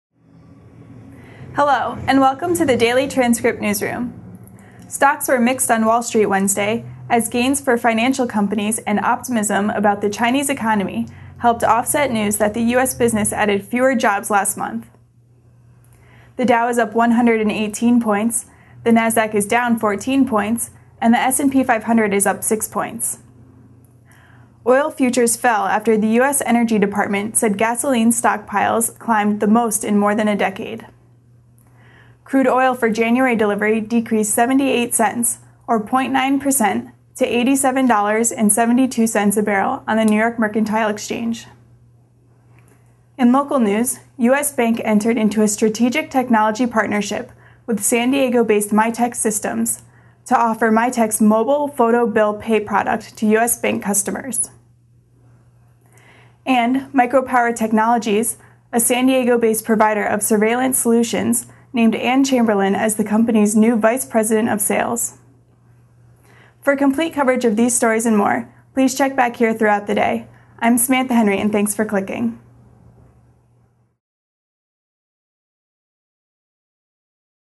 San Diego Business News - Morning Edition (12/5/2012)
A look at some of the early stories that are making the news on December 5, 2012. For these stories and more please visit San Diego Source SDDT.com